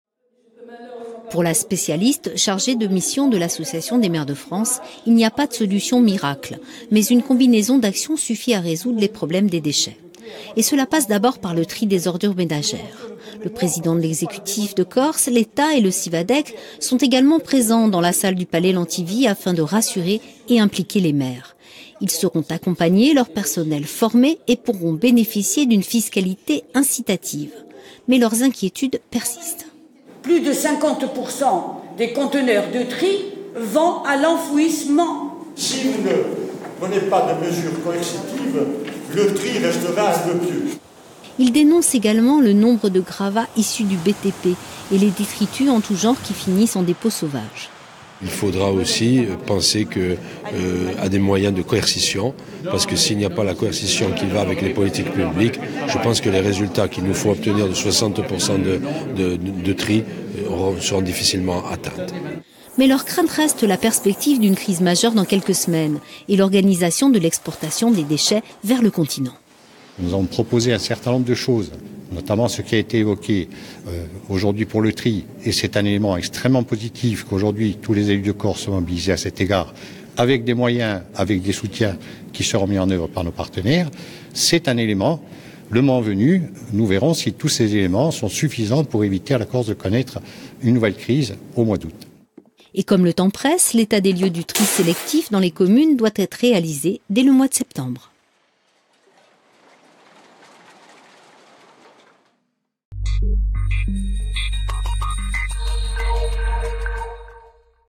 Déchets : les maires de Corse-du-Sud craignent une nouvelle crise
Une réunion s'est tenue à Ajaccio, mercredi, en présence des autorités compétentes et chargées de la mise en œuvre de la collecte et du traitement des déchets. Alors que la menace d'une nouvelle crise reste d'actualité, les réponses apportées n’ont pas forcément rassuré les maires de Corse-du-Sud.

Intervenants  - Aline Castellani, maire de Piana ; Paul -Marie Bartoli, maire Propriano ; Etienne Ferrandi, maire d'Alata ; Xavier Poli, Vice-Président du Syvadec.

Equipe - Sylvie Wolinsky ; Jennifer Cappai-Squarcini ; Caroline Ceccaldi ; Jean-Jérôme Delsol.